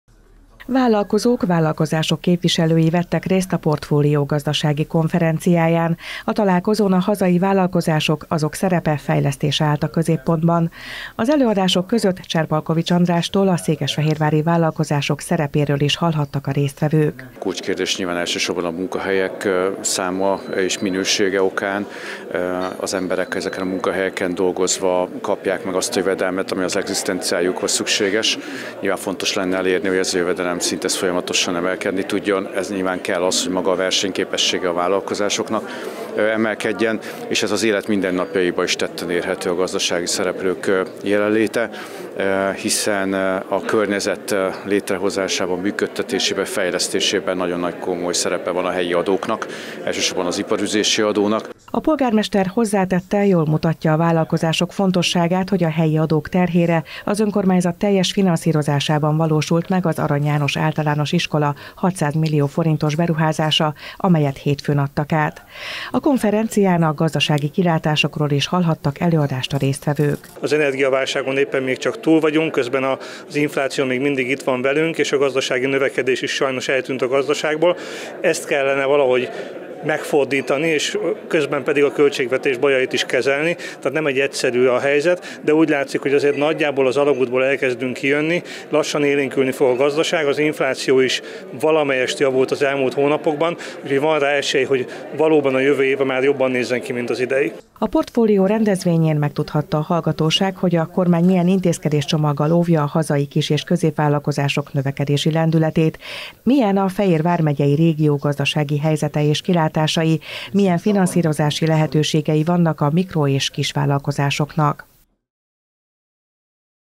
Középpontban a hazai vállalkozások
Milyen szerepe van Székesfehérváron a vállalkozásoknak? Hogyan segíti a kormány a kis és középvállalkozásokat? Hol tart most a magyar gazdaság? Ezekre a kérdésekre is választ kaphattak a résztvevők a Portfolio rendezvényén.